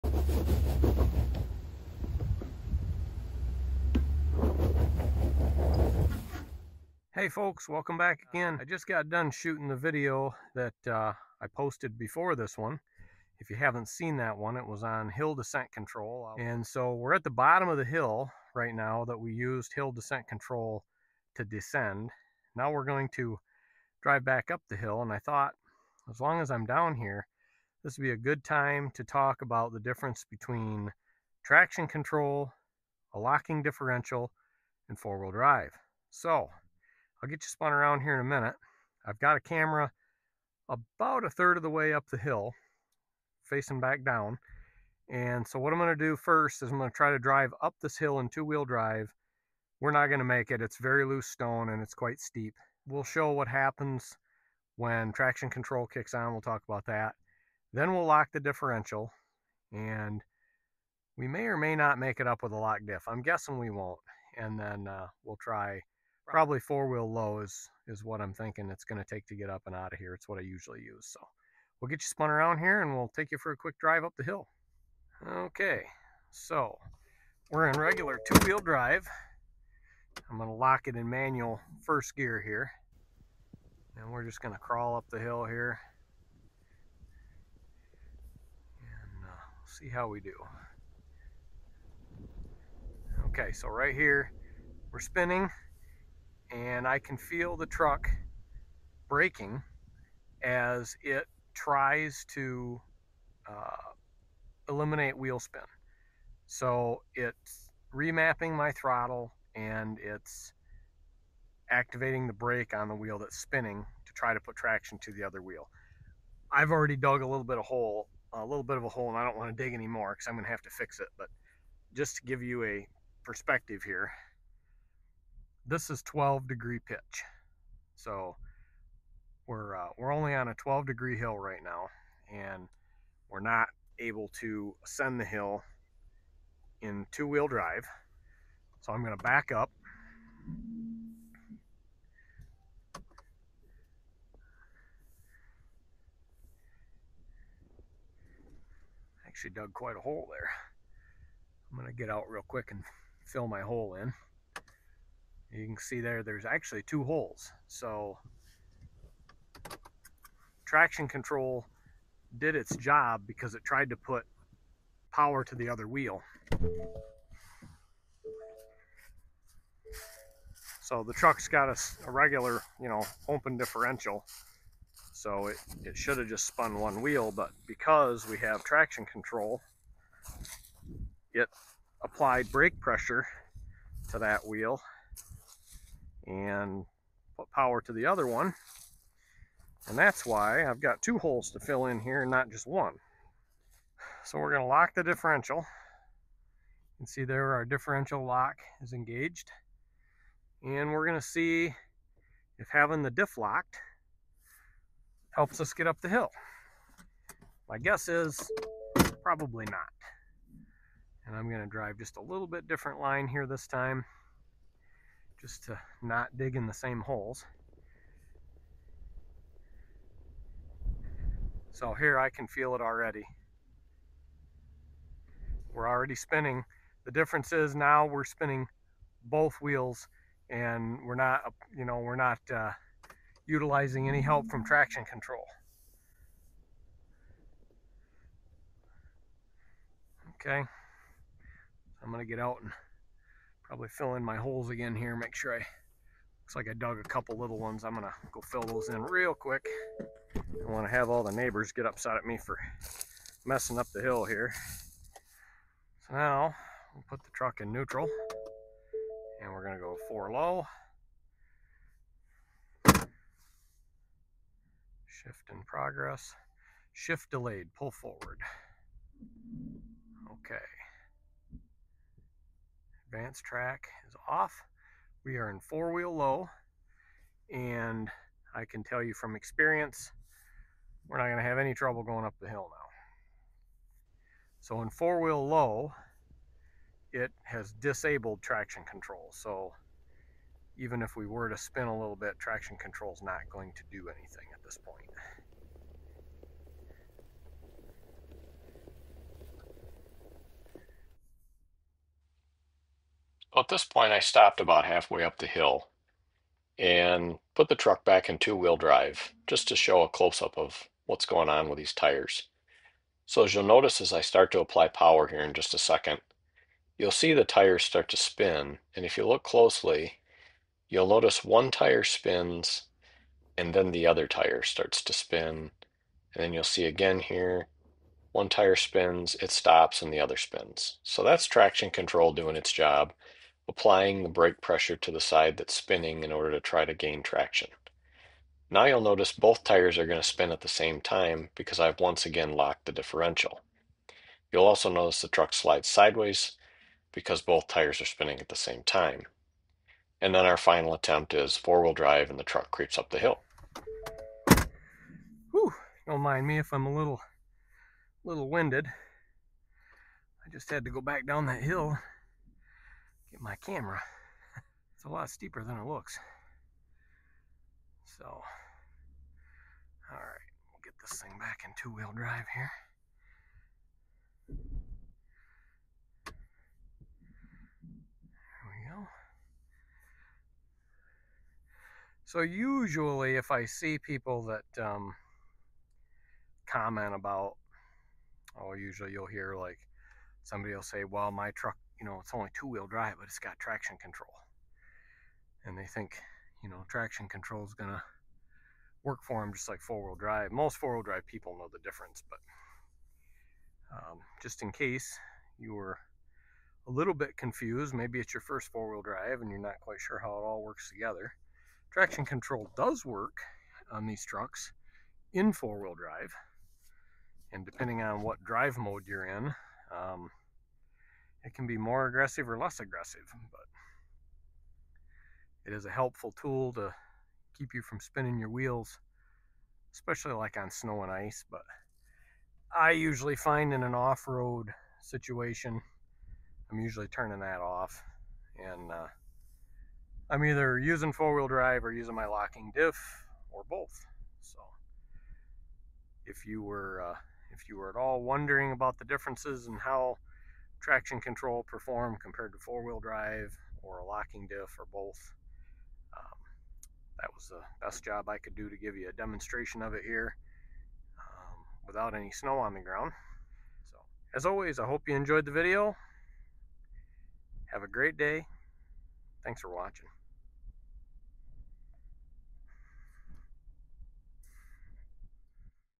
FordSuperduty Traction control vs locked diff vs 4wd
In this video, I use my 2022 Ford Super Duty F350 with the 7.3 Godzilla to demonstrate how traction control works compared to activating my electronic locking rear differential and finally engaging 4 wheel drive low range as I try to climb a steep hill covered with loose stone.